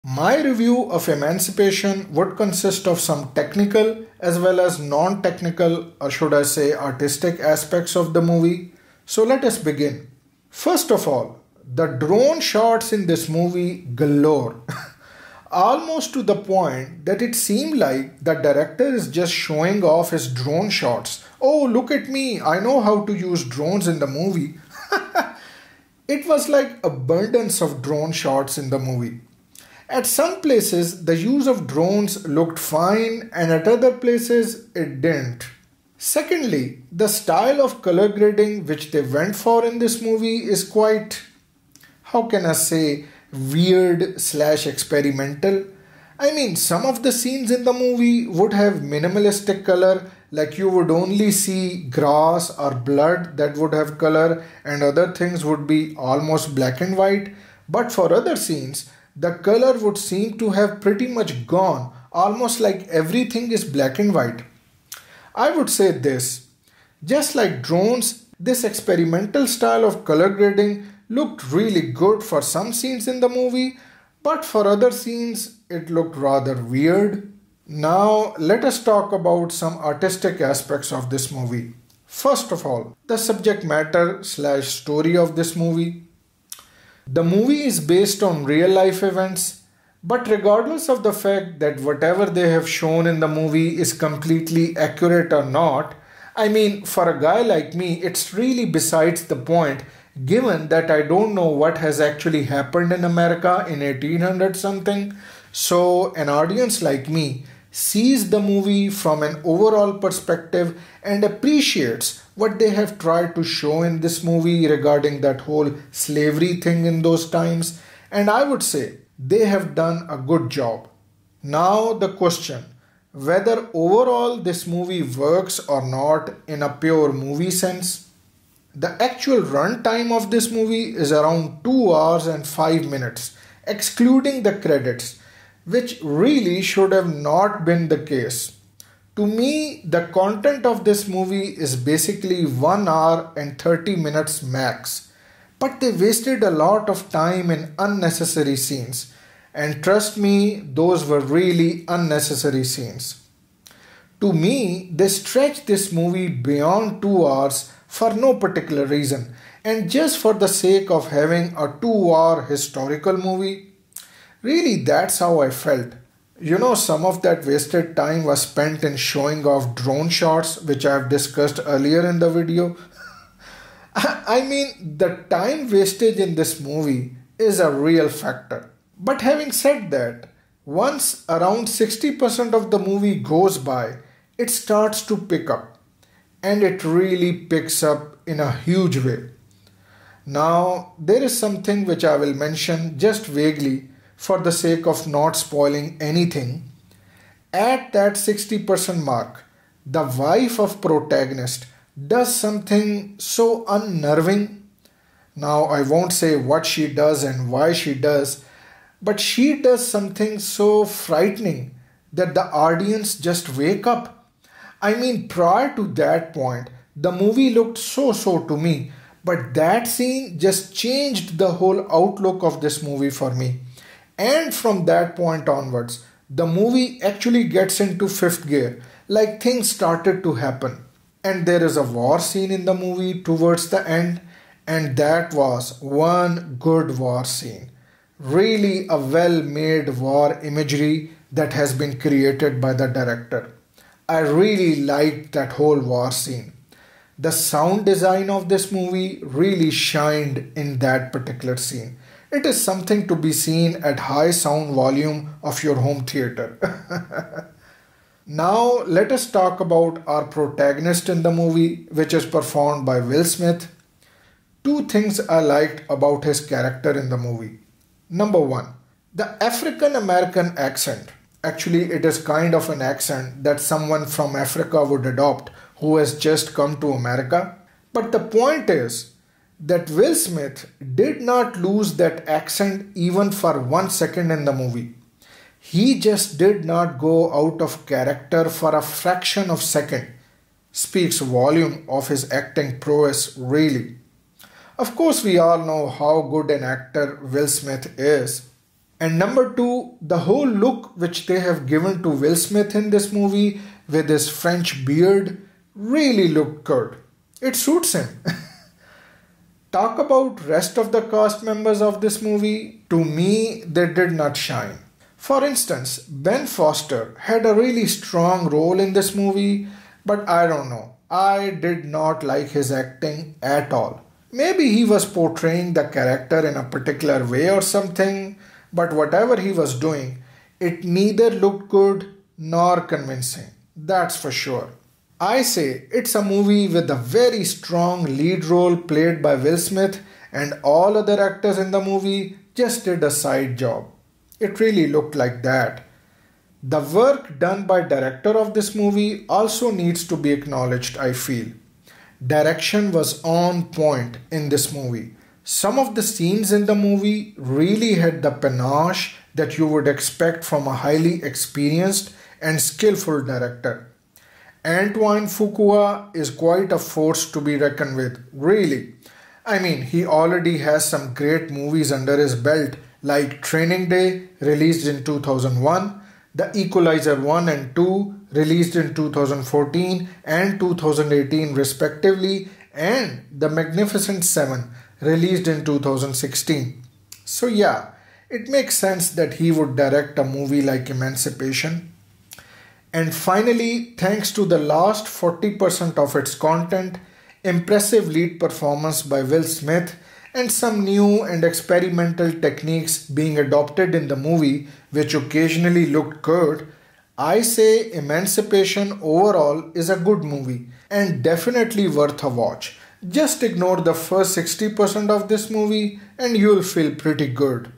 Non Spoiler Review of Movie Emancipation 2022 / Apple TV Plus / Will Smith / Film Impressions
Non Spoiler Review of Movie Emancipation 2022
Apple TV Plus Original
Will Smith
Ben Foster
Film Impressions
Sarah Lifestyle